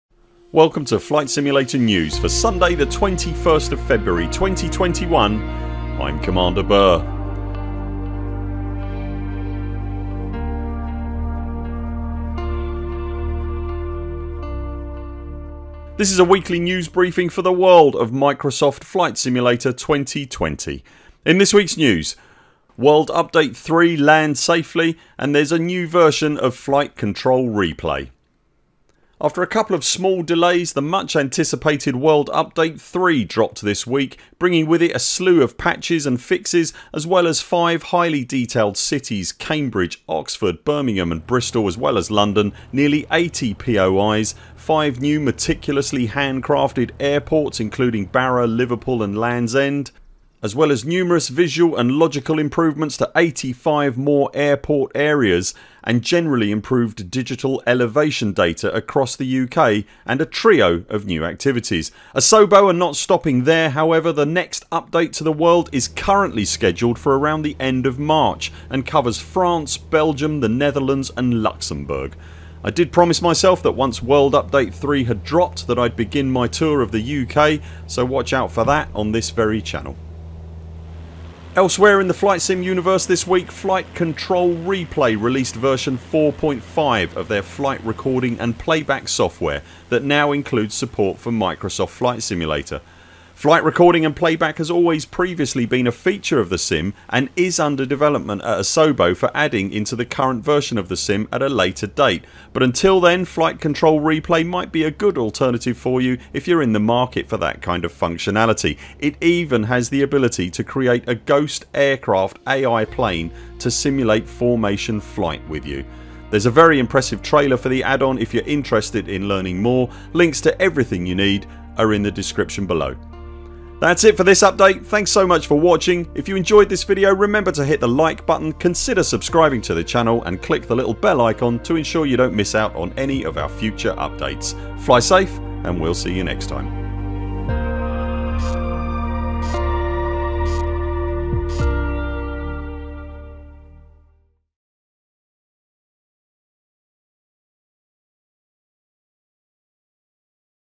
Flight Simulator 2020: World update 3 Lands and FlightControlReplay 4.5 (Out Now) Will Fly with You
In the news this weeks World Update 3 lands safely and There’s a new version of FlightControlReplay.

DONATE directly to the Stream and see your name in lights .. with added Parrot ;)
BUY all things merch at the Buur Pit merch stores
(UK, EU, REST OF WORLD)